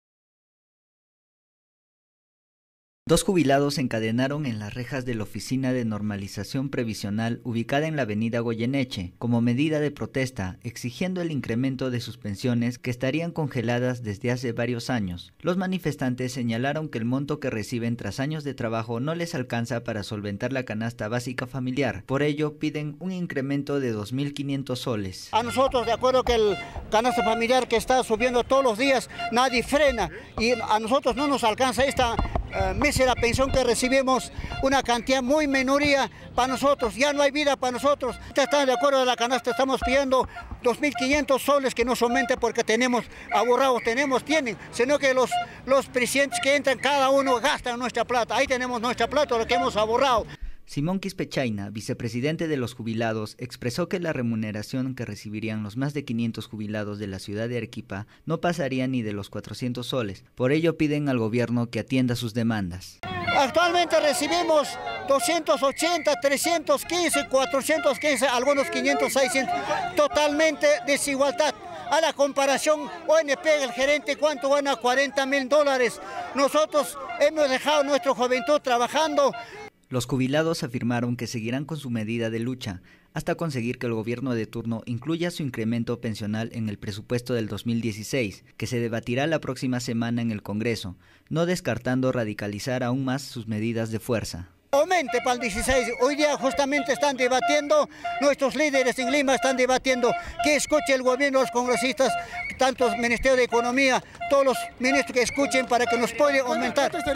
ENCADENAN
DOS JUBILADOS SE ENCADENARON EN LAS  REJAS DE LA OFICINA DE NORMALIZACIÓN PREVISIONAL ONP, ACOMPAÑADOS DE LOS CESANTES  DE LA LEY 19990 Y OTRAS LEYES, PROTESTARON PORQUE NO HAY INCREMENTO DE SUS PENSIONES, A PESAR QUE LA CANASTA BÁSICA FAMILIAR SUBE CONTINUAMENTE.